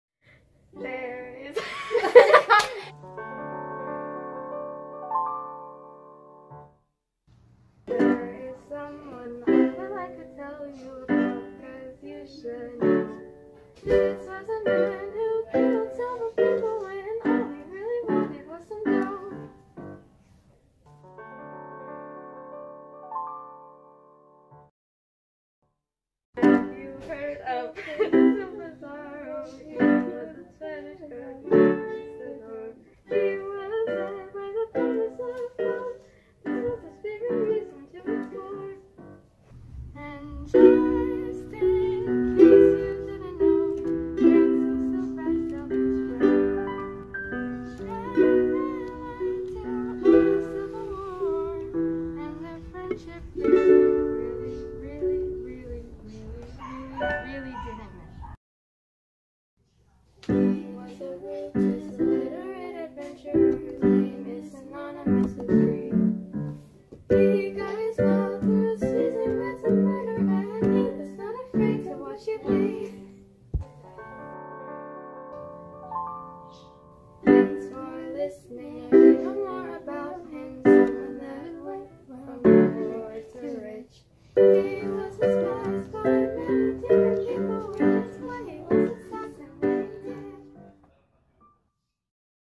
world history song project 2
Verse 1
There is someone I would like to tell you
about, cause you should know
This was a man who killed several people when all he really wanted was some dough

Verse 2
Have u heard of Francisco Pizzaro
He was a Spanish conquistador (con- keys- tah- door)
He was led by the promise of wealth
This was his favorite reason to explore

Bridge
And just in case you didn’t know
Francisco backstabbed his friend
And it led to a civil war
and their friendship really really really really
really really really didn’t mend

Verse 3
He was a ruthless, illiterate adventurer
Whose name is synonymous with greed
He got his wealth through seizing, ransom, murder
and was not afraid to watch you bleed

Verse 5
thanks for listening,
now you know more about him
someone that went from poor to rich
He was despised by many different people
Which is why he was assassinated

Thesis: The explorer Francisco Pizarro is a zero because he was ruthless, driven by wealth, illiterate adventurer whose name is synonymous with treachery, greed, and annihilation, from acquiring wealth through kidnapping, ransom, and murder of the native populations he encountered.

Paragraph (explanation)
The explorer Francisco Pizarro was not a good man. This song proves this stance. In each verse, it focuses on the perspective of the natives. It shows that his ultimate goal was to become wealthy, and because of this he did unacceptable things to achieve his goals. He even backstabbed his supposed friend, which led to a Civil War known as the Spanish conquest of the Inca Empire.